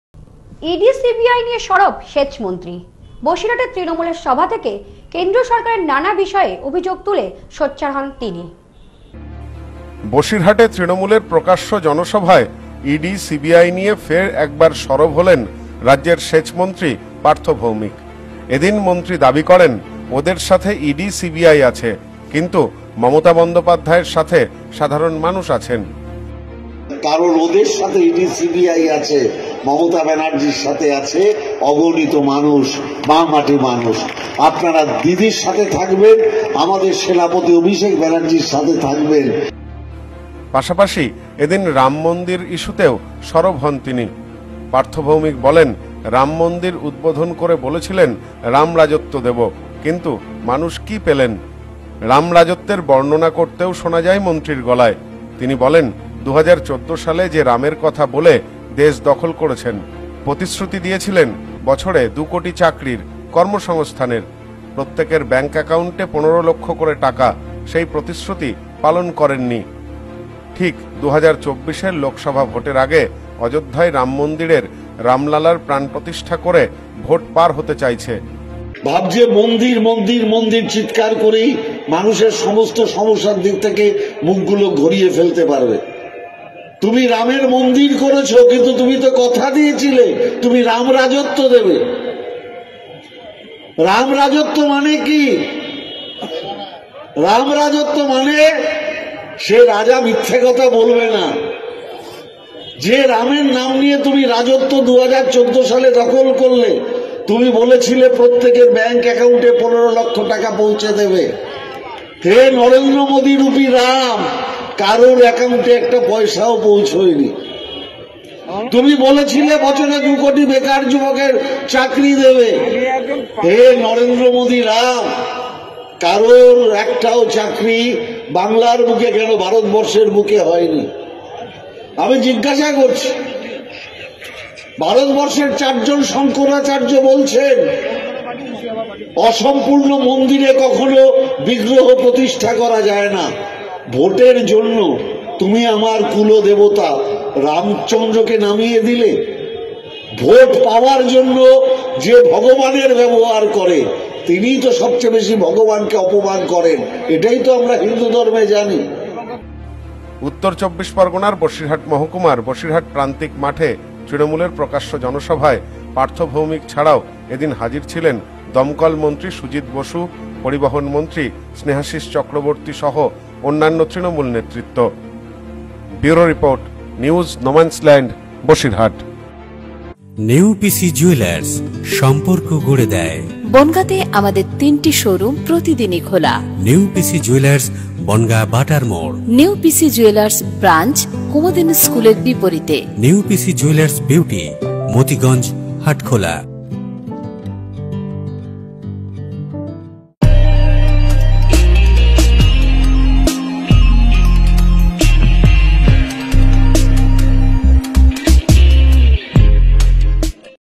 ইডি-সিবিআই নিয়ে সরব সেচমন্ত্রী।
ইডি-সিবিআই নিয়ে সরব সেচমন্ত্রী। বসিরহাটে তৃণমূলের সভা থেকে কেন্দ্র সরকারের নানা বিষয়ে অভিযোগ তুলে সোচ্চার হন তিনি।